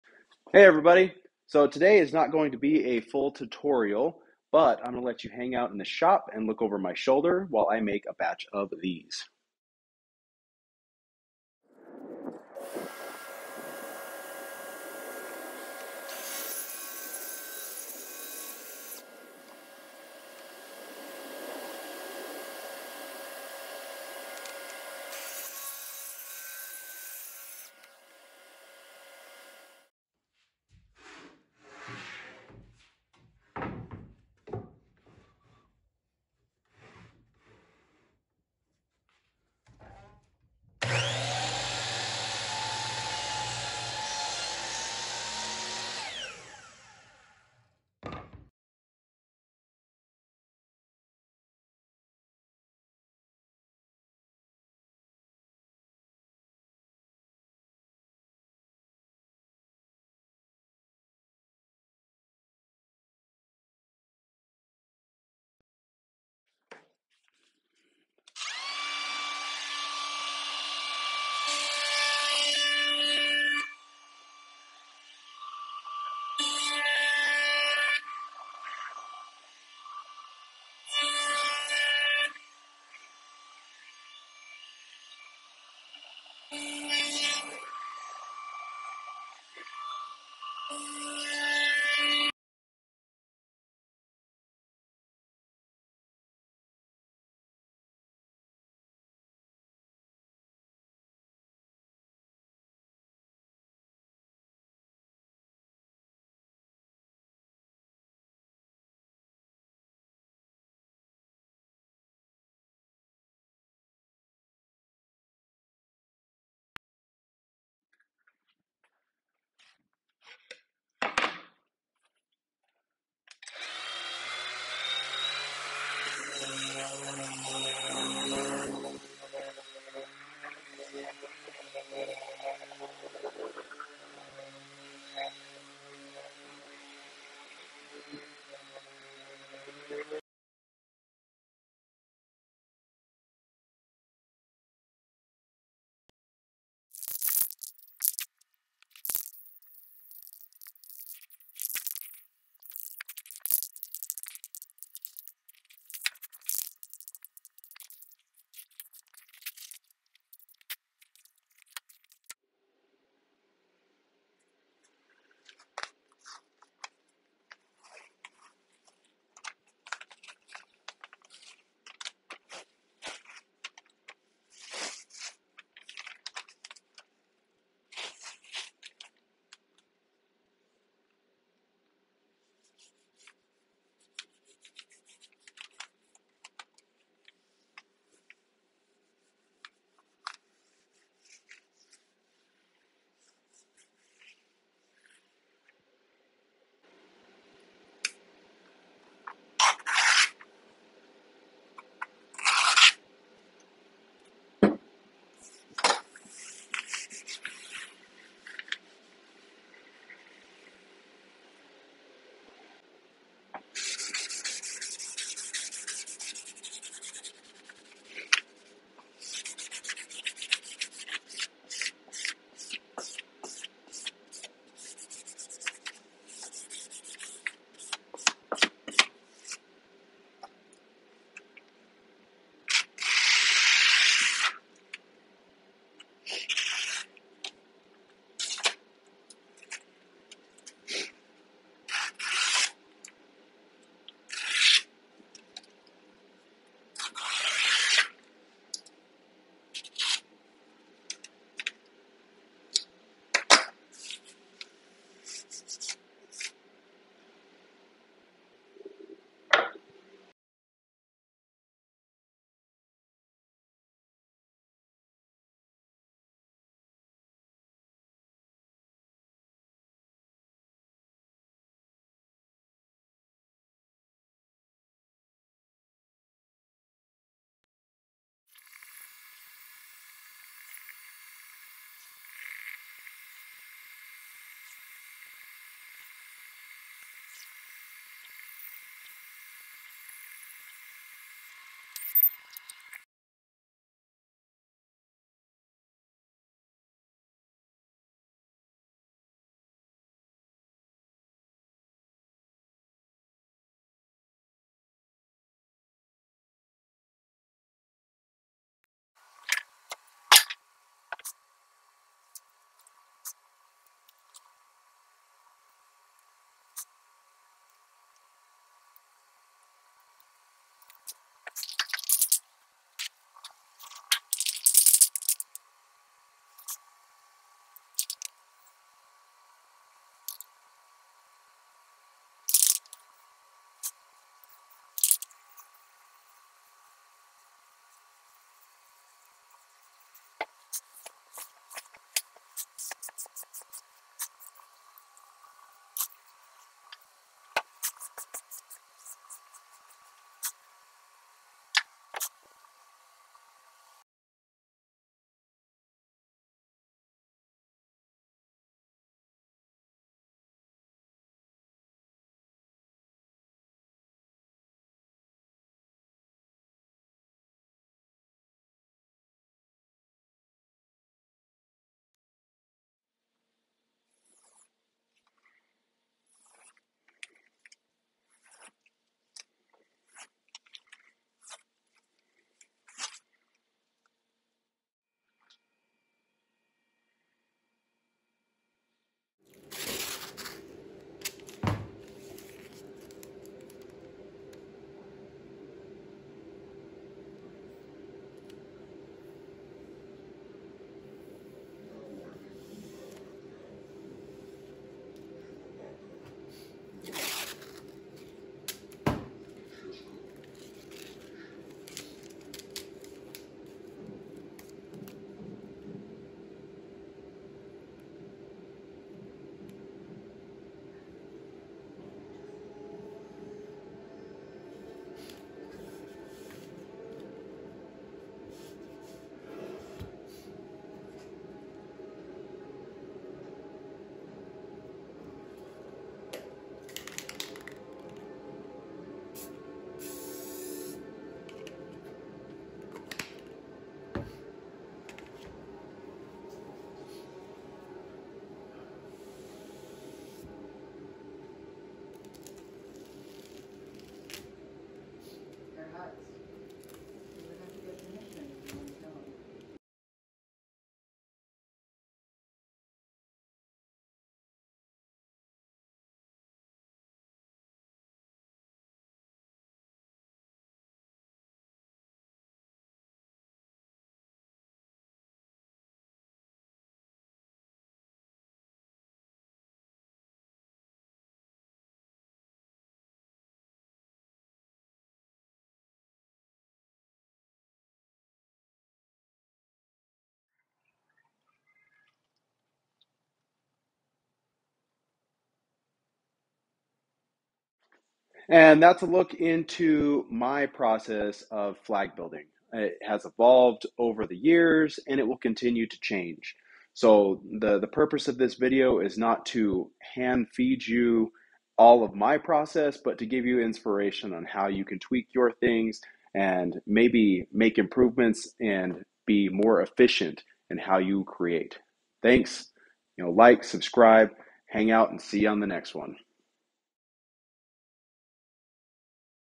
Flag Making! Lasers, power tools and paint!
This video is an over the shoulder view of my wooden flag making process to help inspire you to find improvements in your current workflow.

Lightburn Course
Course.beardedbuildsco.com/store

Amazon.com/shop/beardedbuildsco

Mask Tape:

Socials:
Instagram.com/beardedbuildsco

Facebook.com/beardedbuildsco